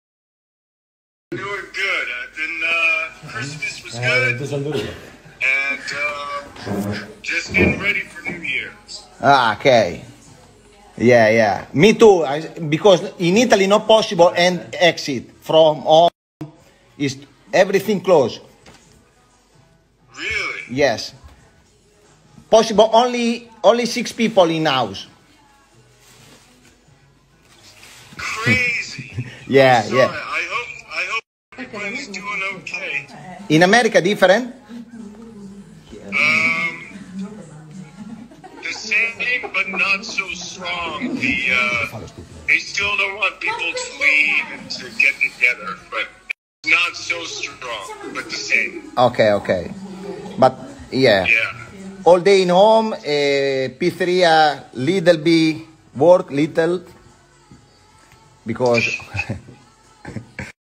Keanu Reeves Having Face-time with His Italian Biker Friend & Talking About Covid, Lockdown,New year
اگر ویدیوهای روزانه  کیانویی میخواید و دوست دارید در جریان اخبار کیانو ریوز قرار بگیرید، سابسکرایب کنید و اینستاگرام و تلگرام مارو دنبال کنید: